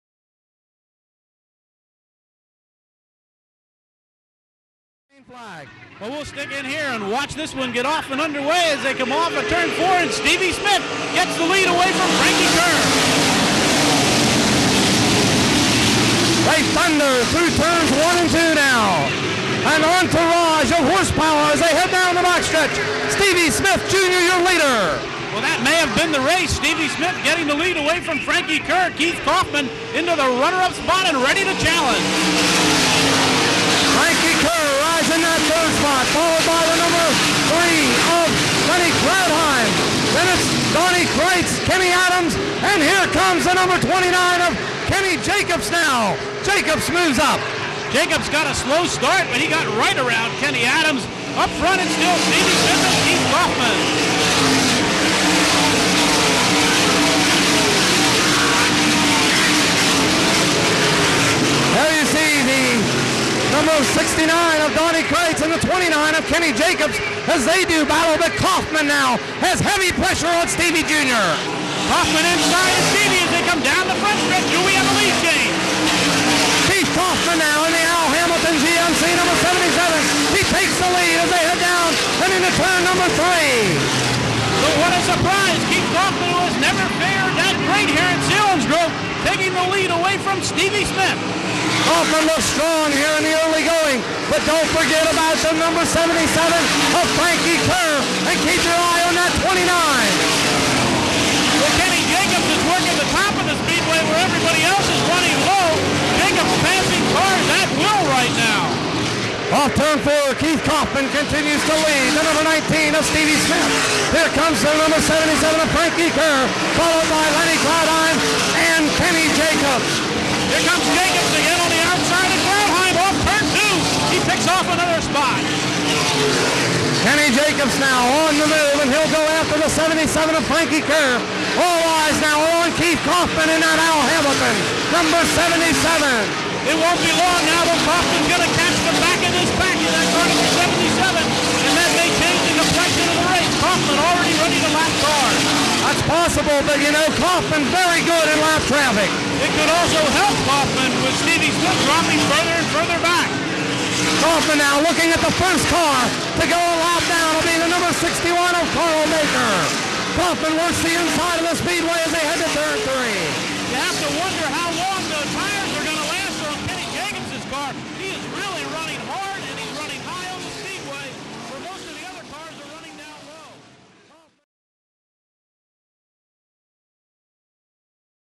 Classic 6th annual Selinsgrove Speedway National Open sample clip
This is a short clip of the 6th annual 10,000 dollar to win "Blaise Alexander Sprint Car National Open" at Selinsgrove Speedway held on Oct. the 1st ,1988    Donnie Kreitz was back in true form, setting fast time in his familure # 69 sprinter, Kreitz won this race in 86. Keith Kauffman was also back,  back behind the wheel of the Al Hamalton # 77 . Keith had just ,the night before won the Port Royal Speedway point championship for the 8th time in his career. Neither  Dave Blaney nor Maynard Yingst were on hand, the only other past winners of the race. Blaney won it in 87 and Yingst the 83, 84, and 85 winner.  Ken Stahl and Johnny Strawser call the lap by lap action and there is a lot of it.  Bobby Landis crashes in the 1st heat, Bobby Strawser hits the fence and then gets wacked by Joe Geartie in the 4th heat, Brian Seidel and Bobby Parrow get locked togather and go out over turn three in the B-Main. and to cap off the night Graig Whitecomb gets t-boned and rolls his Street Stocker in turn one during there main event . this DVD captures a great part of Selinsgrove Speedway History order DVD # 10188 e-Mail for details We accept Pay Pal and US Postal Money Orders.